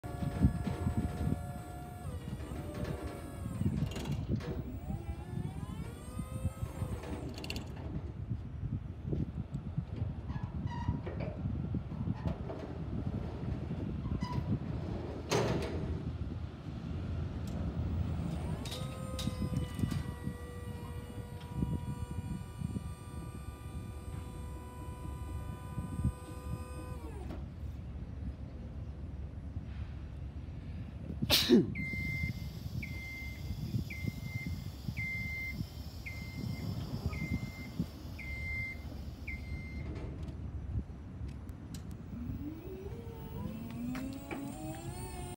STOCK #20551: 2016 GENIE GS-4047 ELECTRIC SCISSOR LIFT FOR RENT IN SAN DIEGO -- OTAY MESA SALES
STOCK #20551: THIS 2016 GENIE 40' LIFT GS-4047 ELECTRIC SCISSOR LIFT IS PART OF THE OTAY MESA SALES, SAN DIEGO & SOUTHERN CALIFORNIA AERIAL WORK PLATFORM RENTAL FLEET. THIS SAN DIEGO RENTAL ELECTRIC SCISSOR LIFT FEATURES A 46' WORKING HEIGHT, AN ONBOARD CHARGER, DECK EXTENSION, POWER TO PLATFORM AND NON-MARKING CUSHION TIRES. CALL 844-OTAYSALES FOR MORE INFORMATION ON THIS OR ANY OF OUR SAN DIEGO AWP RENTALS.